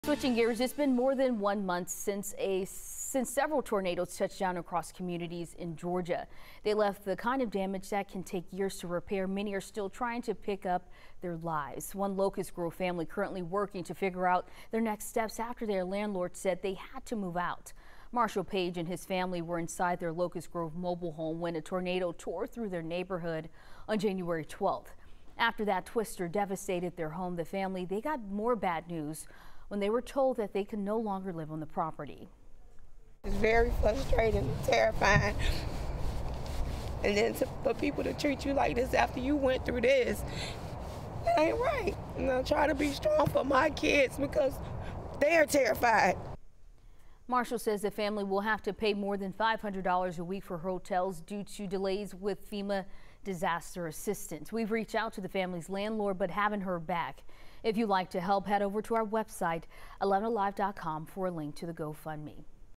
Henry County family of 12 given 7 days to move out of tornado-damaged home
The Page family said their landlord only gave them a week to move out, all the local shelters are full, and FEMA assistance is still processing.